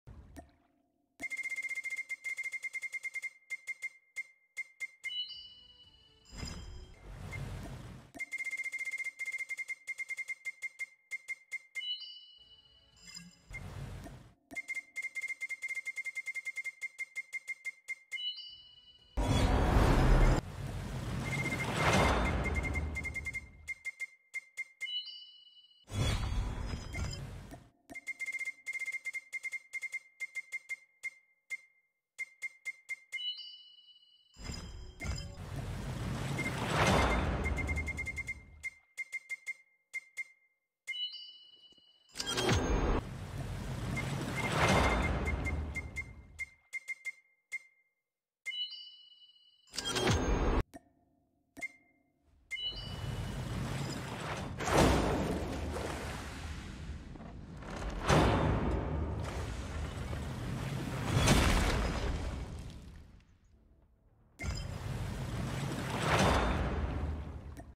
Buying full Anchors Away Draw! SVD Admiral Star and Dame Captions Quip | Legendary SVD Draw
Please Join My WhatsApp Group I Will Update you and everything About CP Price.